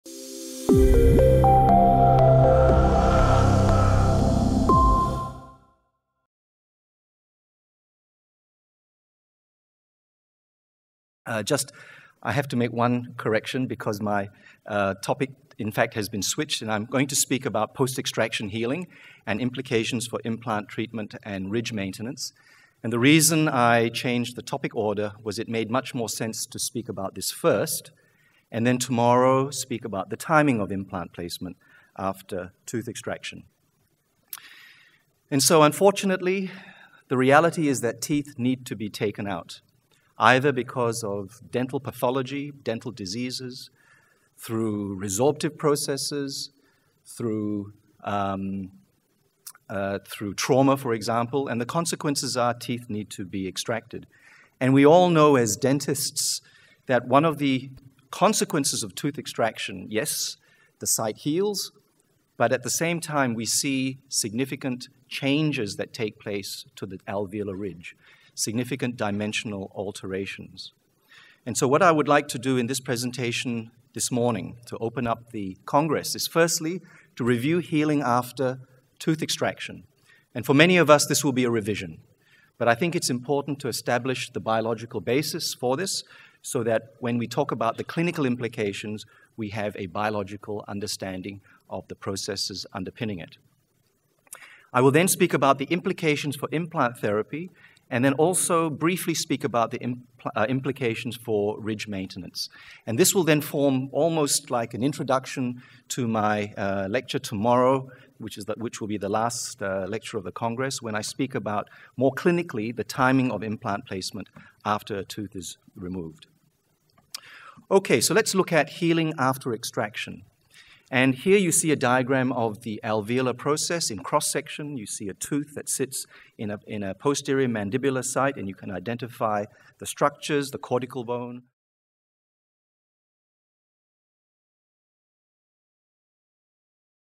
Post-Extraction Healing – Implications for Implant Treatment and Ridge Maintenance | Stephen Chen
ITI Academy Congress Lecture Preview: Post-Extraction Healing – Implications for Implant Treatment and Ridge Maintenance by Stephen Chen.